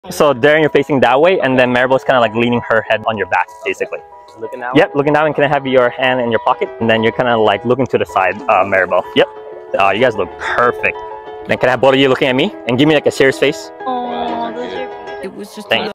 When your wedding photographer gives CLEAR posing instructions
One of the top concerns a couple has, is that they’re worried about posing so as a team, we make sure that we help the couple get as comfortable as possible so all the photos and videos are more natural and genuine. We walk them through each and every pose and help move things around if necessary. Our videographers, assistants, and photographers know how to work well with each other so that the photoshoots go smoothly and efficiently. We want to make this a great experience for every couple we work with. Thank you to the 300+ couples who have chosen us so far for your special wedding day!